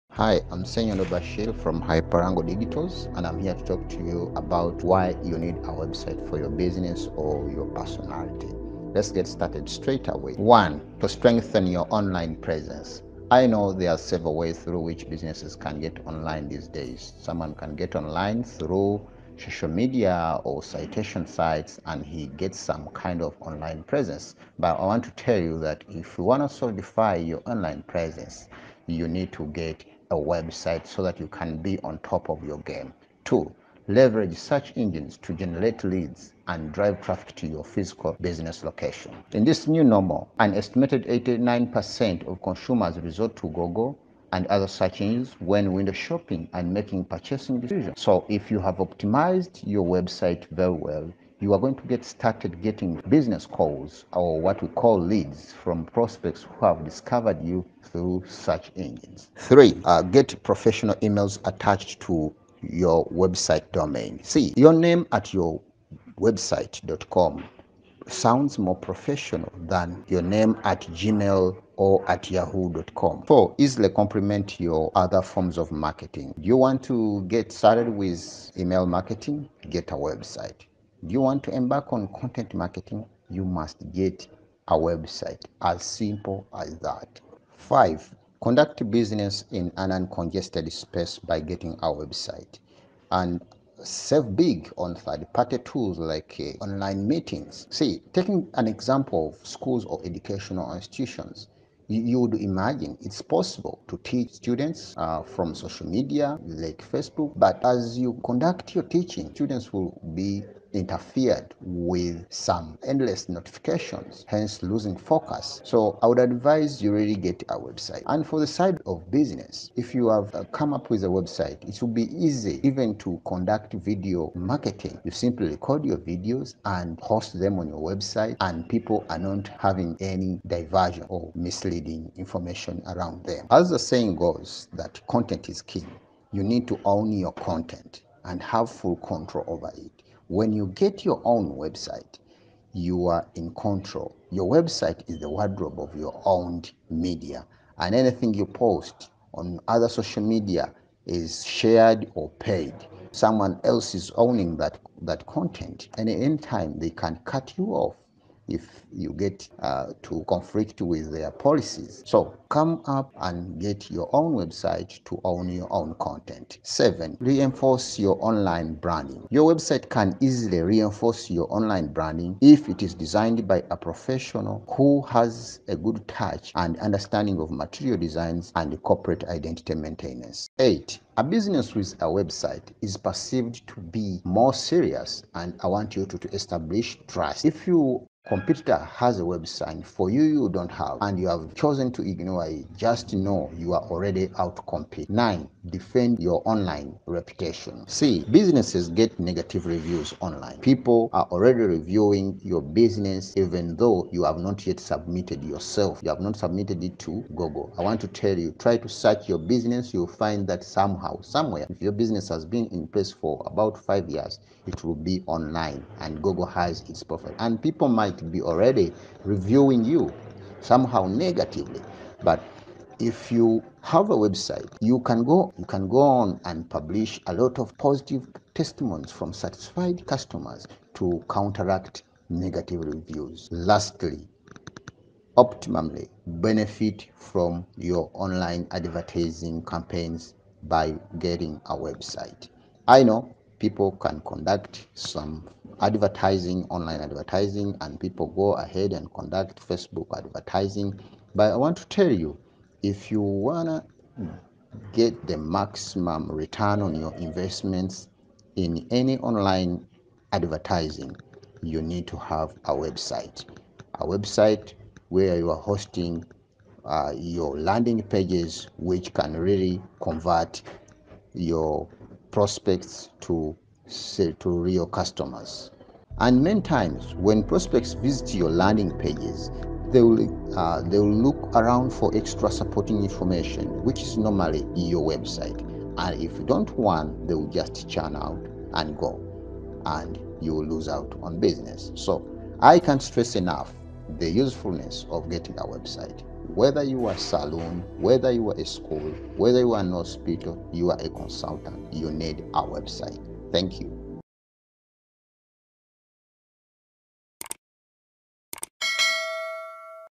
10 Reasons Why you need a website for your business full video
Hello, here's a video with 10 reasons why you need a website for your business or personality, celebrity, or institution.
Produced by a Ugandan digital marketing expert; Ssenyondo Bashir.

More than ever before, in this new normal - post COVID19,  online presence is a must and a website is one solution that you can't ignore when building a strong online presence. Watch this 5-minute video to appreciate why?